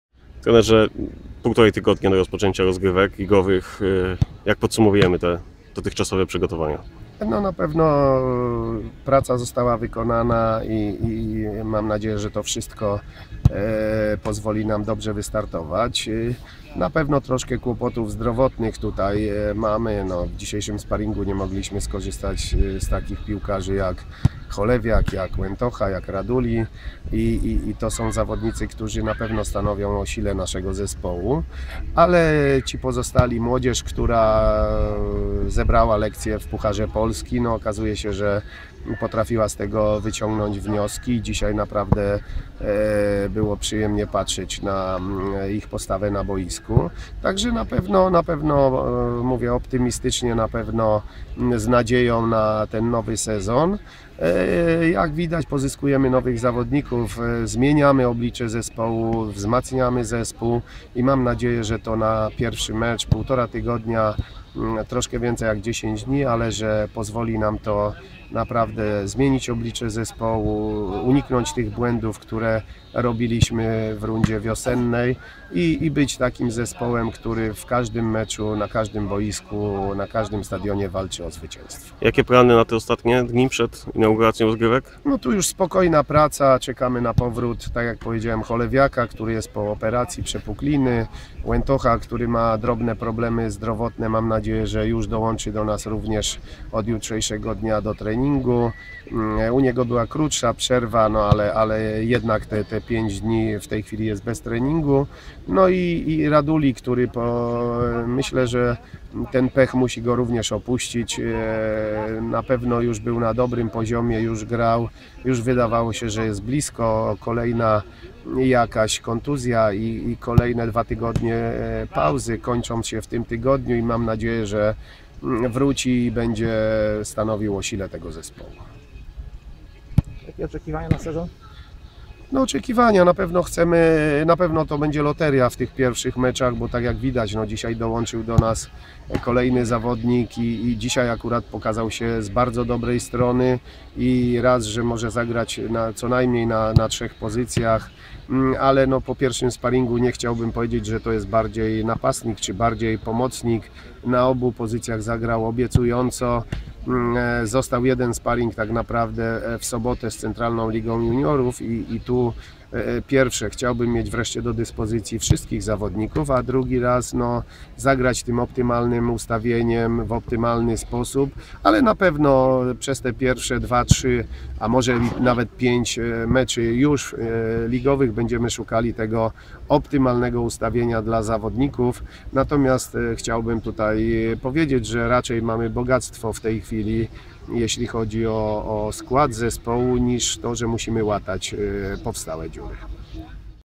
Trener Stali Mielec po sparingu z Siarką Tarnobrzeg
Trener Stali Mielec po sparingu z Siarką Tarnobrzeg - Stal - Siarka 3:0, 22 lipca 2015